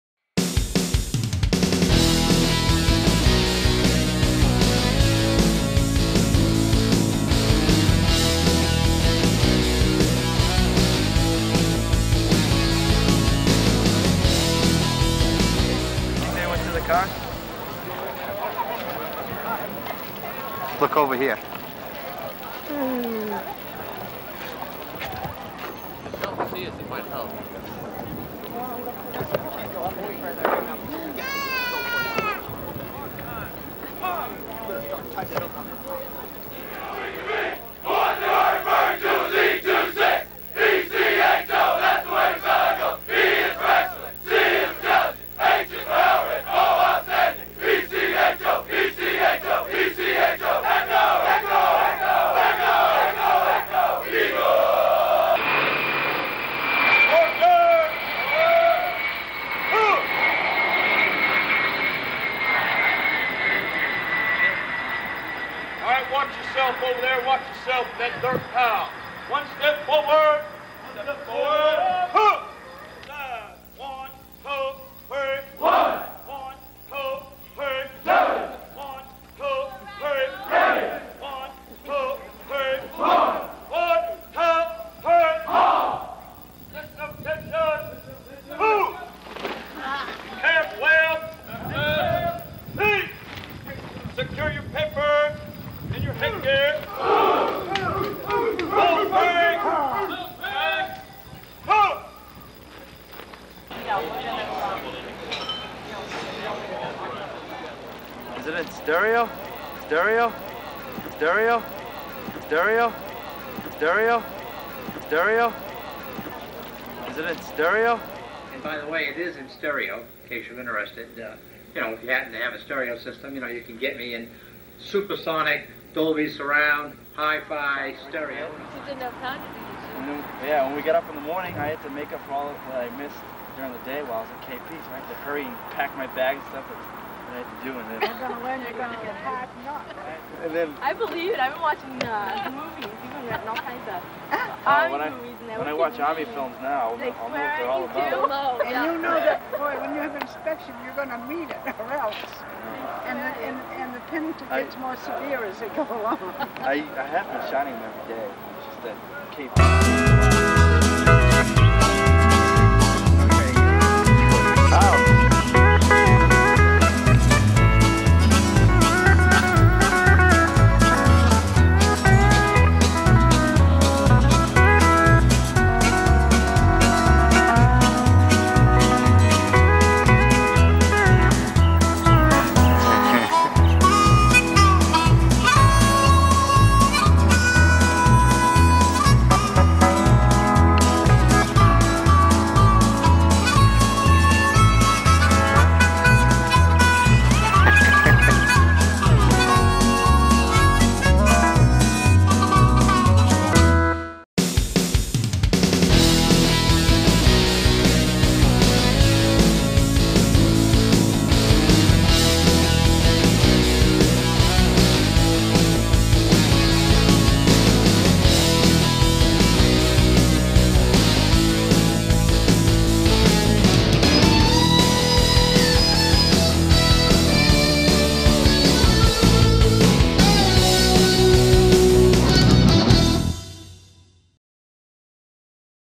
Fort Dix, NJ Graduation Day October 1987 US Army Basic Training E - 1 - 26 2nd Platoon
Video of my graduation from ARMY basic training. Family drove  to Ft Dix, New Jersey to see me graduate, October 1987. My dad video taped this